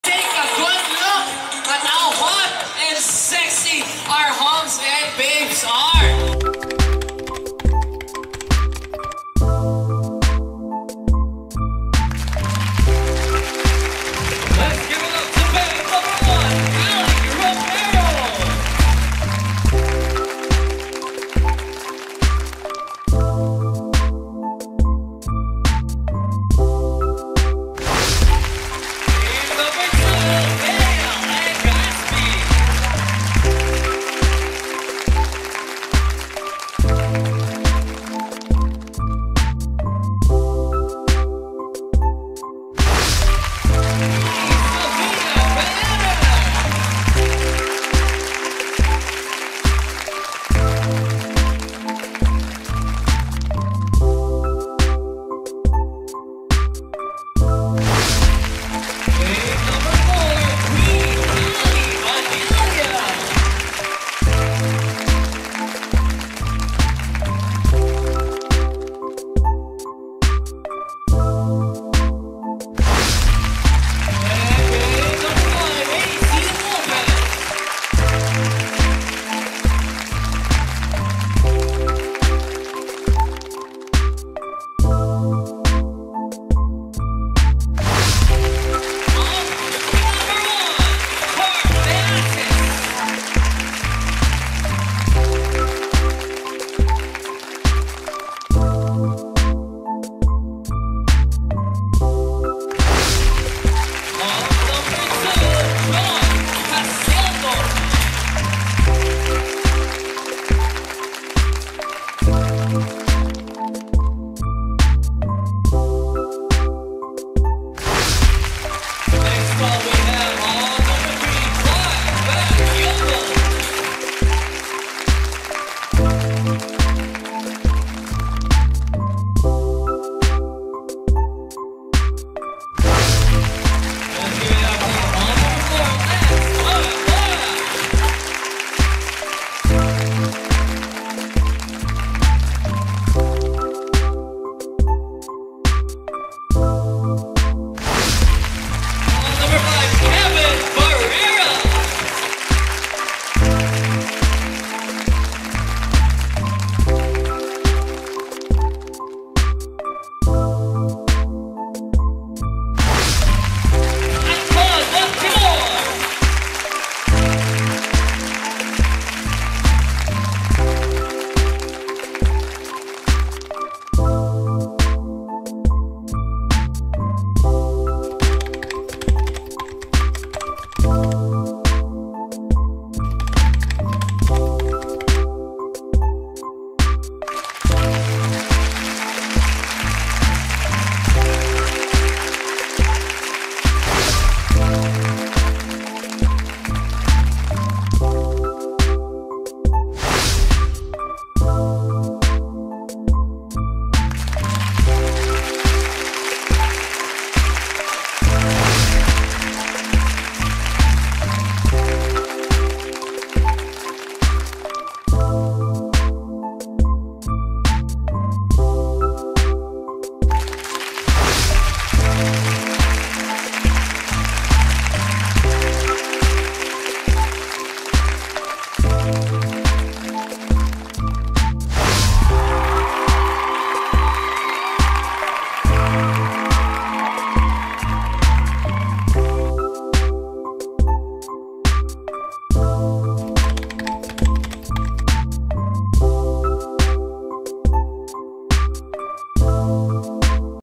Summer Bodies Quest 2024, 11th Elimination - Swimwear Segment
The hunks and babes in the 11th elimination of 'Summer Bodies Quest 2024' in their sizzling swimwear.